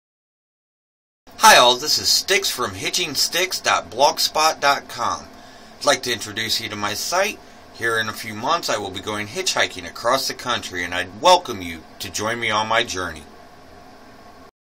Welcome to HitchingStiks.Blogspot.com
Welcome to my blog here in a few months I will be hitch hiking across the country and I welcome you to join me on my journey